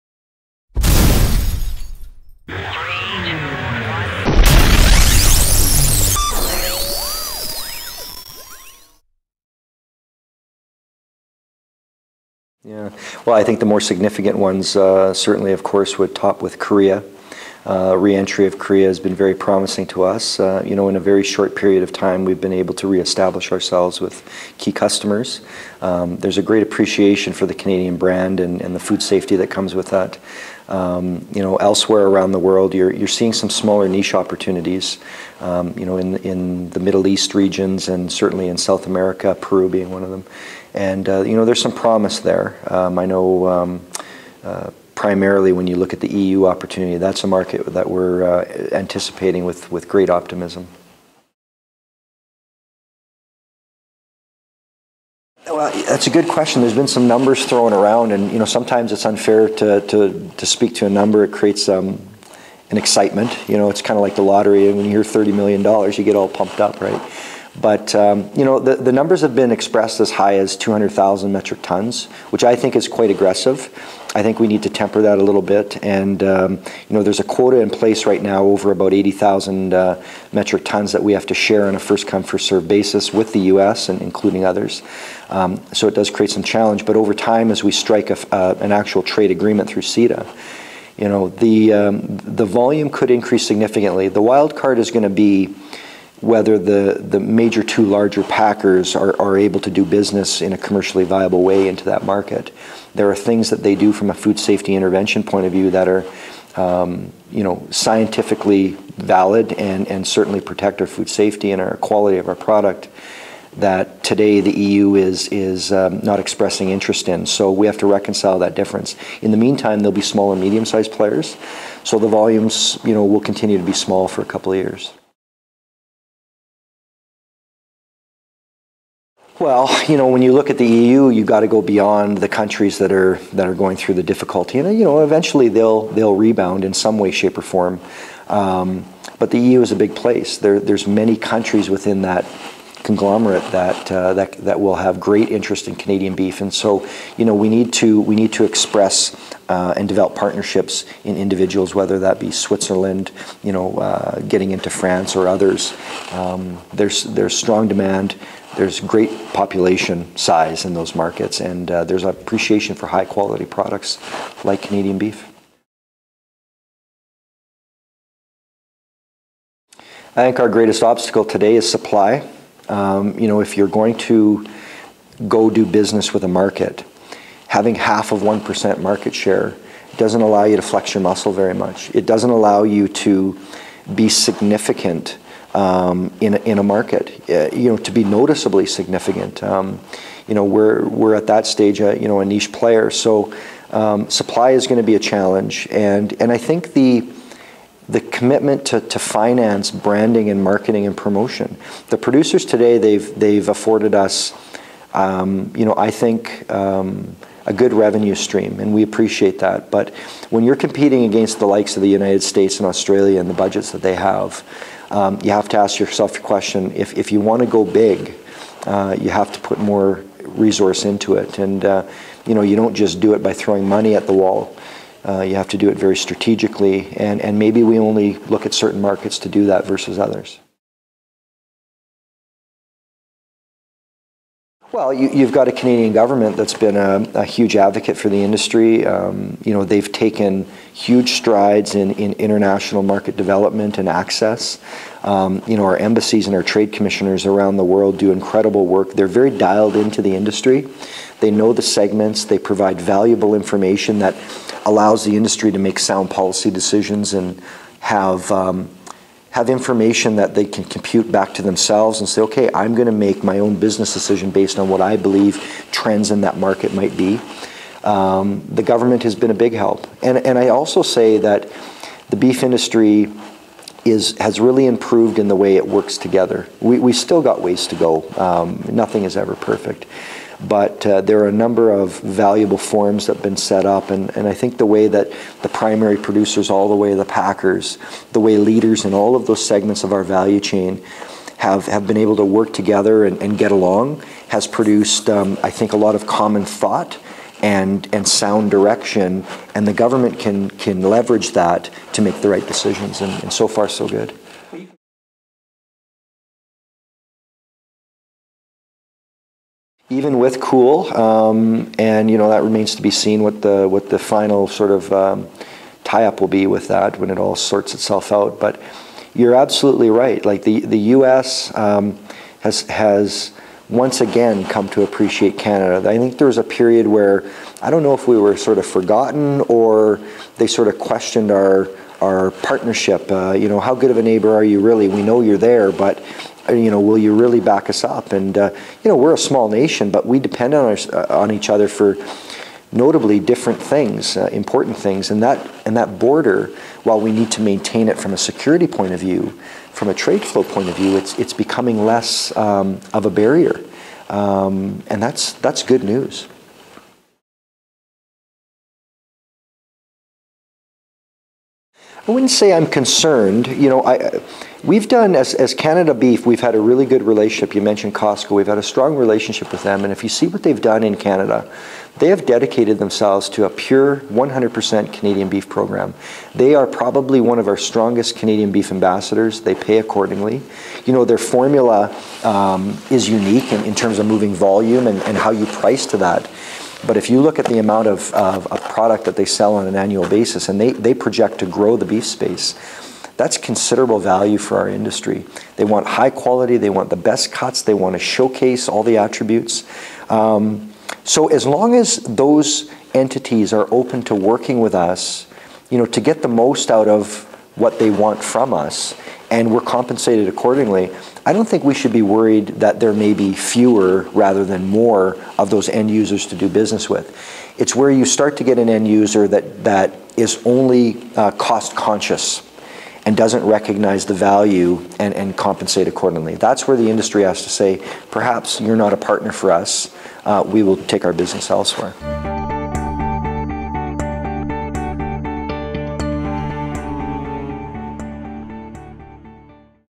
Robert Meijer on International Market Openings & Canada's Beef Trade Relationships
Robert Meijer, President of Canada Beef Inc. talks about the Canadian beef trade and the issues surrounding it.